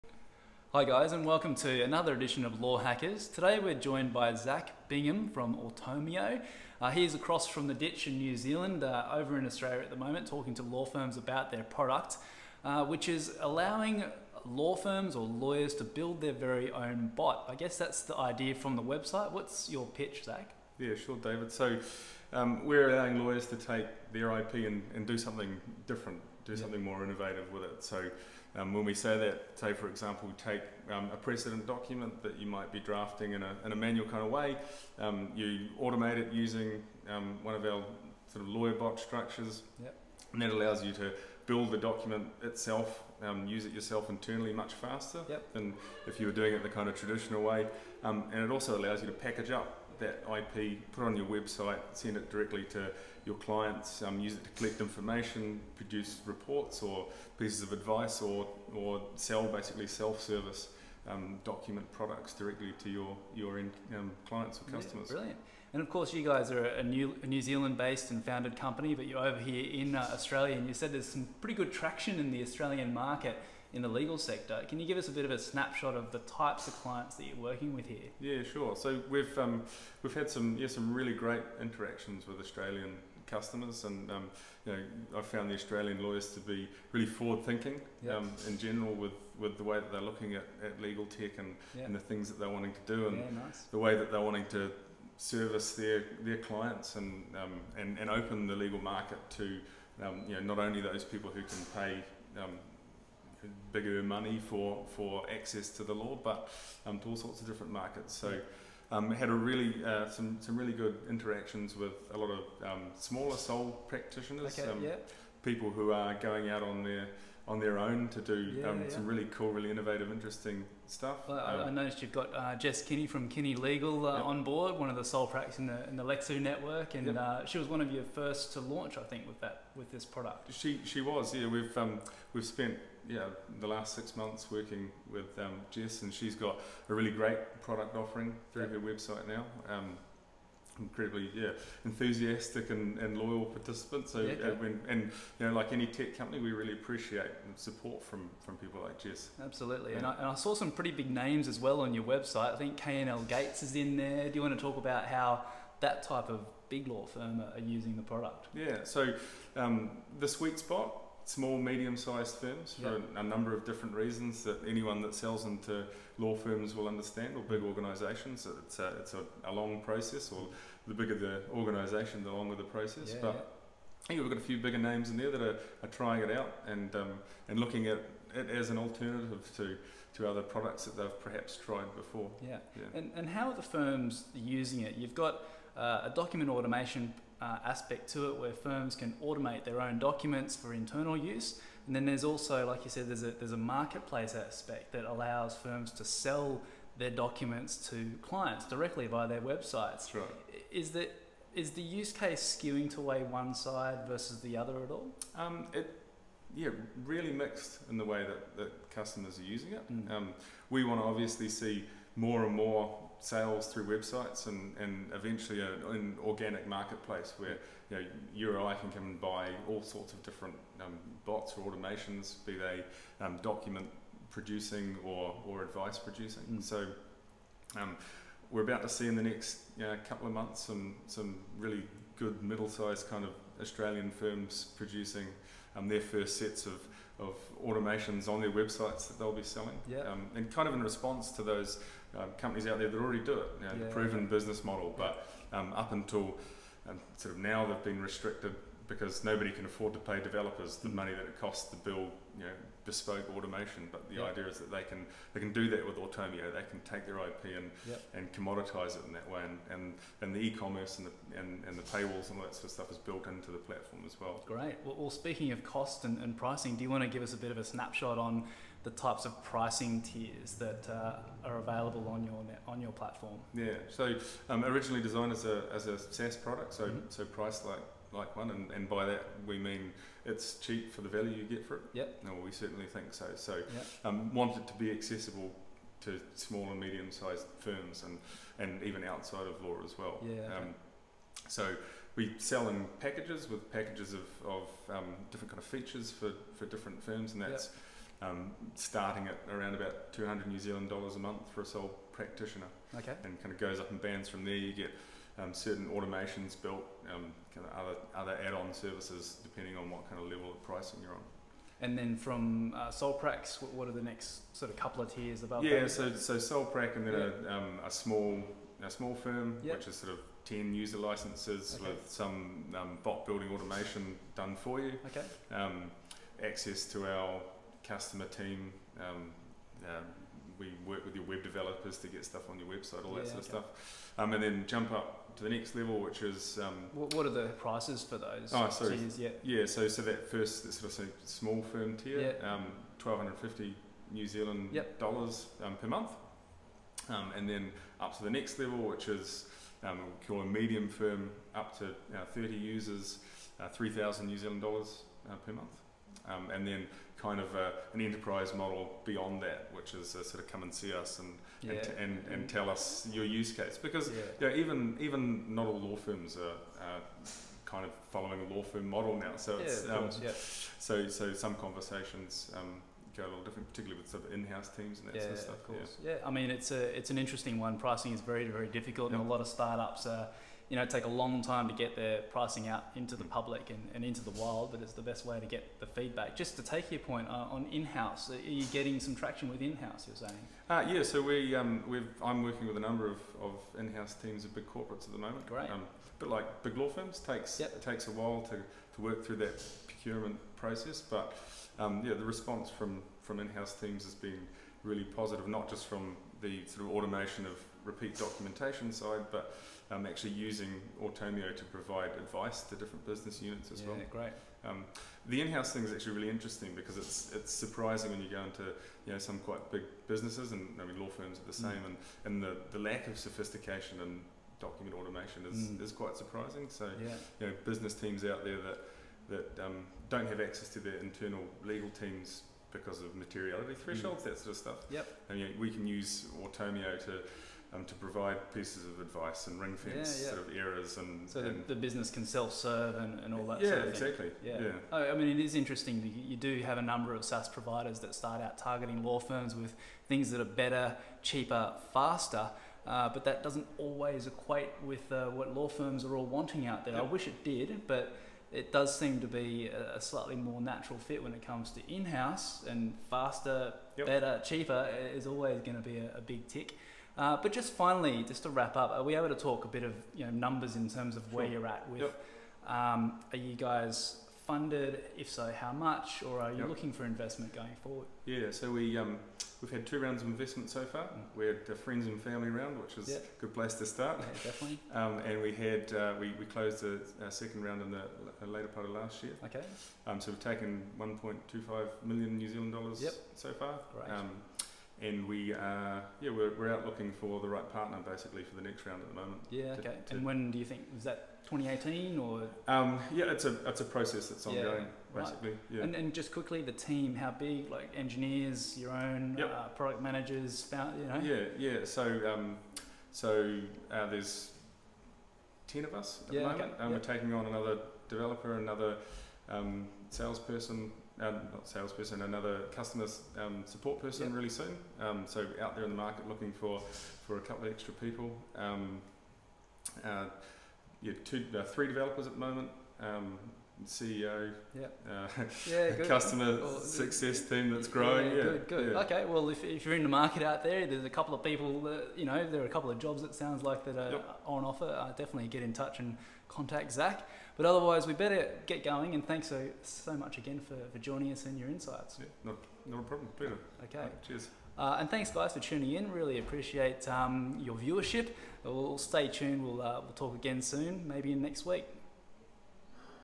Zac Bingham, BDM at Automio
Automio BDM, Zac Binghm talks us through their next generation document automation platform for law firms to draft better, faster and cheaper (and monetise their precedents!).